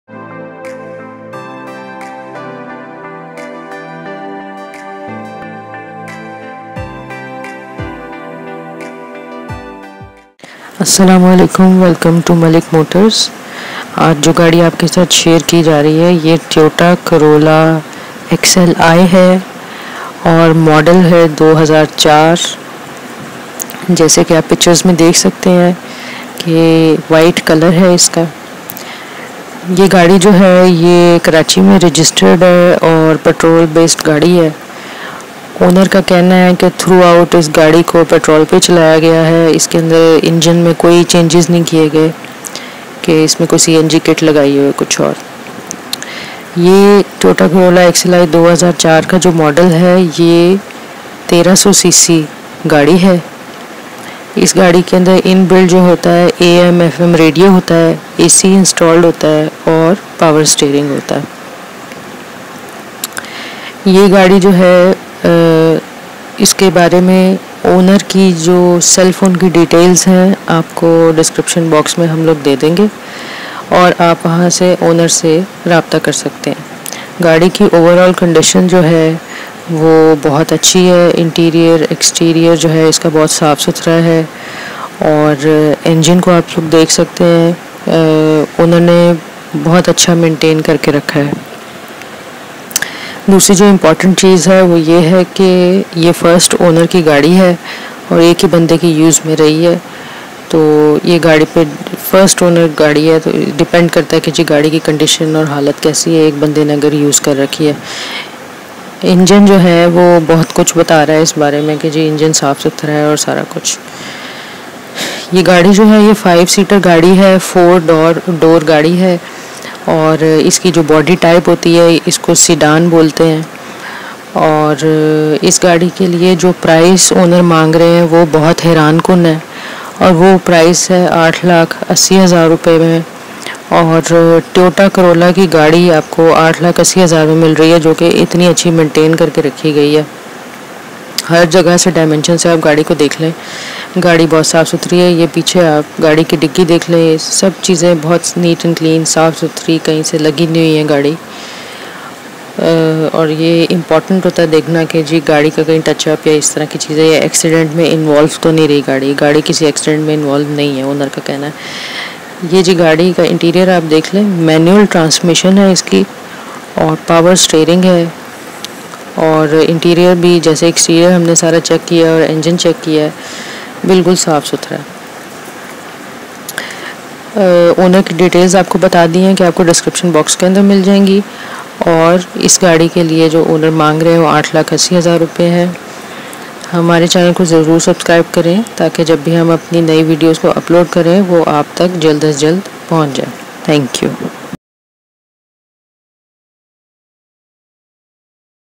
Toyota Corolla XLi |2004 Model | For Sale In Karachi | the malik motors
Make
Toyota

Model
Corolla XLI

Year
2004

KMs driven
12,345 km

Fuel
Petrol

Condition
Used

Registered in
Karachi

Owner details :